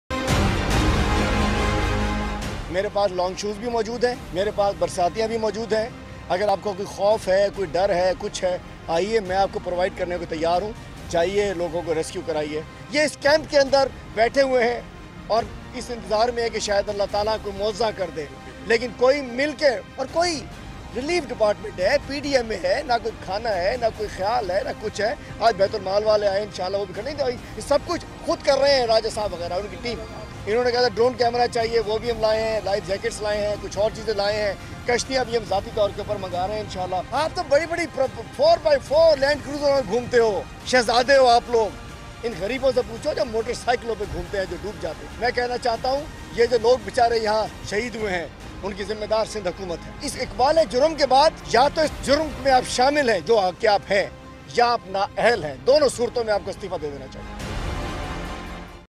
کراچی کی تباہ حال صورت حال کو بہتر بنانے کے بجائے حکومتی نمائندے اپنی سیاست چمکانے میں مصروف
Life HD news  keeps you fully updated of the latest
 and major happenings in Pakistan and around the globe
# Life News T.v